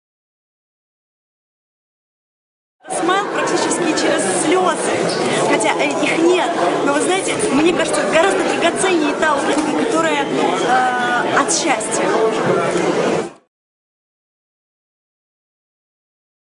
Джамала об улыбке
Джамала после выступления в финале отбора на Евровидение 2011
26.02.11